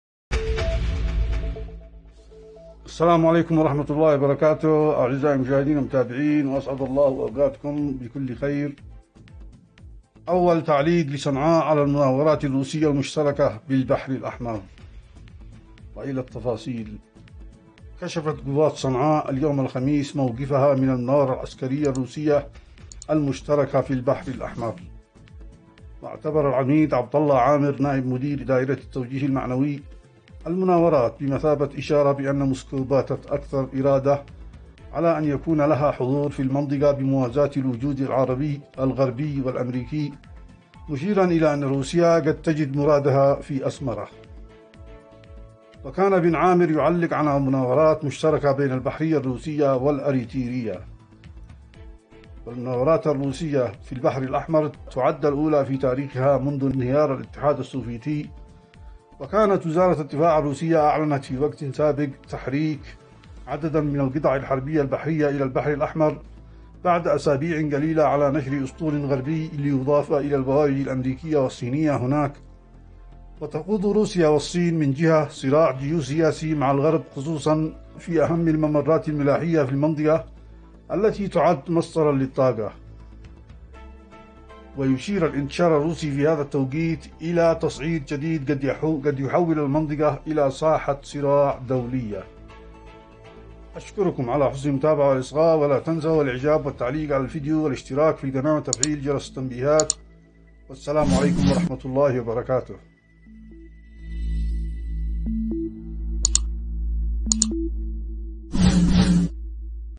هذا هو تعليق صنعاء على المناو رة الرو سية المشتركة في البحر الأحمر
أول تعليق لصنعاء على المناورات الر وسية المشتركة في البحر الأحمر

مسلسل ماء الذهب
اشتباكات_ البحر_الأحمر
مواجهات_ البحر_الأحمر